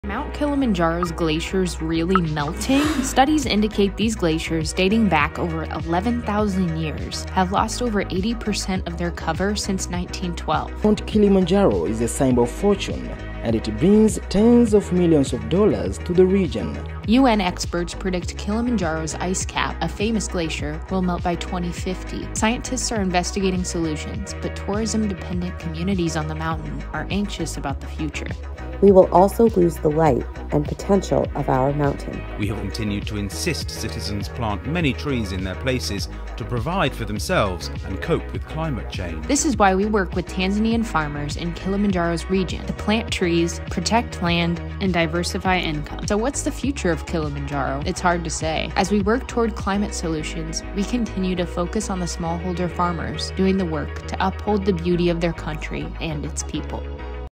Ice Caps on Kilimanjaro
What will be the fate of the glaciers that have historically covered the massive and iconic landmark of Mount Kilimanjaro? It's hard to say. However, the farmers we partner with remain focused on restoring land and preserving the beauty of their country and its people.